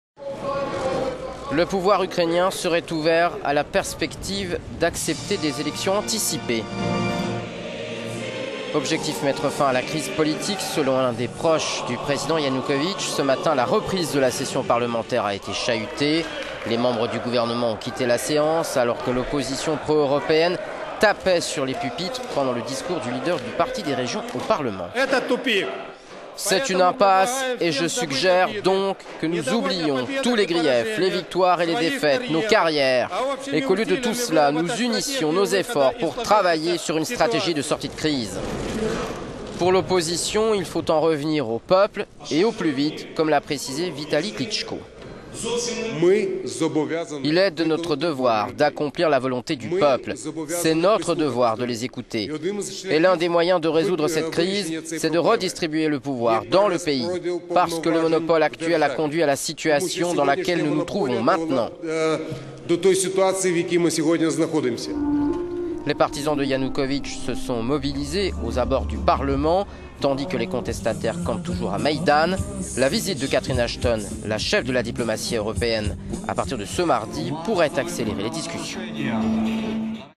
Ukraine : Ianoukovitch pourrait accepter des élections anticipées
Le pouvoir ukrainien serait ouvert à cette perspective pour mettre fin à la crise politique selon un de ses proches. Ce matin, la reprise de la session parlementaire a été chahutée. Les membres du gouvernement ont quitté la séance alors que l'opposition pro-européenne tapait sur les pupitres pendant le discours du leader des Parti des Régions au parlement.

"C'est une impasse, et je suggère donc que nous oublions tous les griefs, les victoires et les défaites, nos carrières, et qu'au...